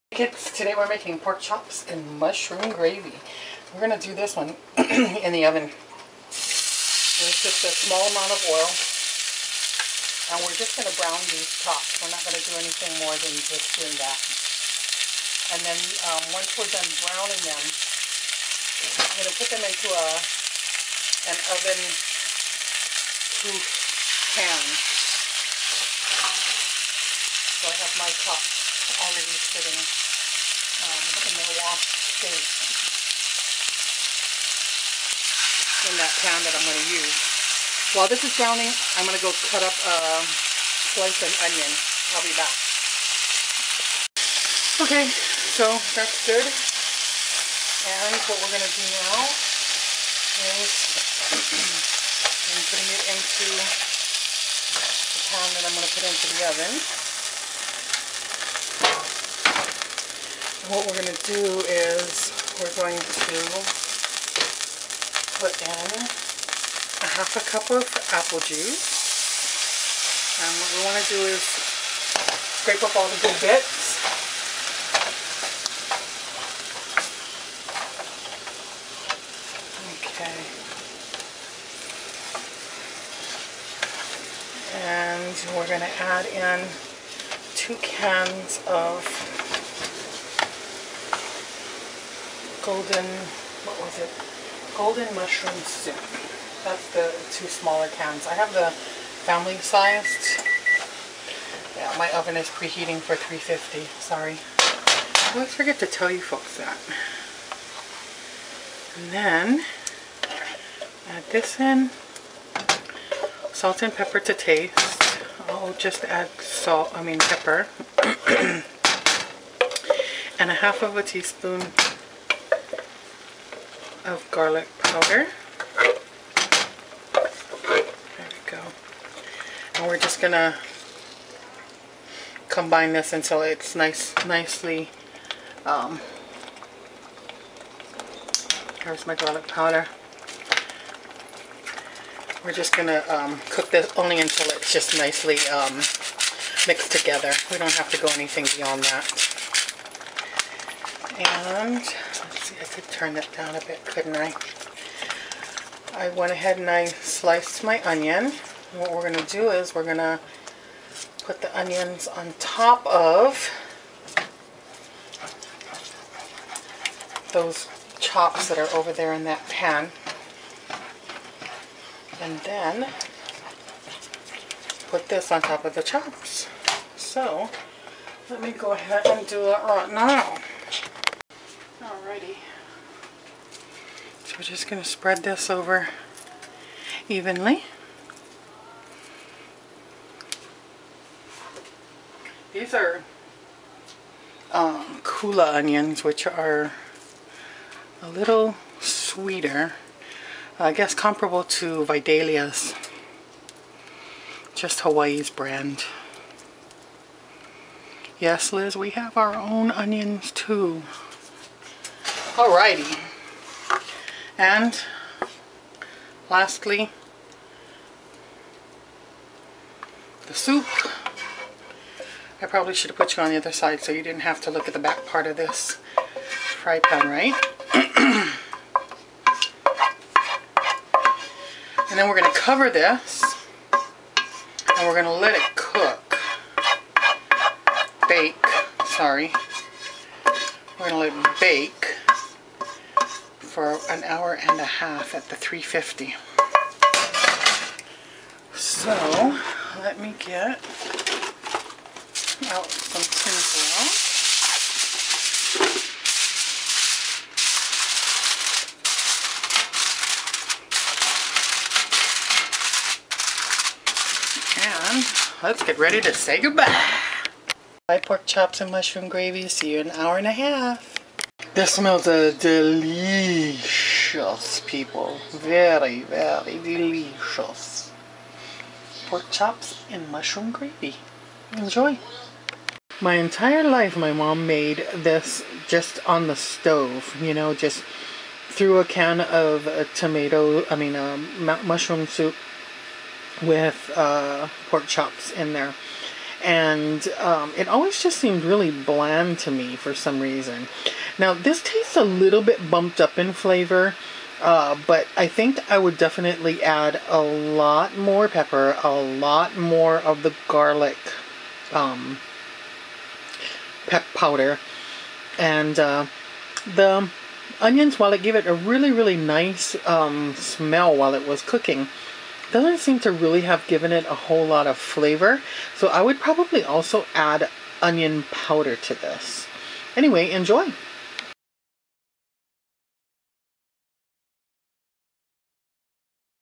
Pork Chops in Mushroom Gravy
6 loin pork chops
1 med. onion, sliced
2 cans Campbell's Golden Mushroom soup
1/2 c. apple juice
Salt and pepper to taste
1/2 tsp. garlic powder

Brown pork chops in small amount of oil. When brown, remove from pan and put in oven proof pan. Add apple juice to pan and cook and stir scraping up brown bits. Add golden mushroom soup and mix well. Stir in salt, pepper and garlic.

Spread sliced onion over pork chops. Pour gravy over it all. Bake covered in 350 degree oven about 1 1/2 hours until pork chops are tender.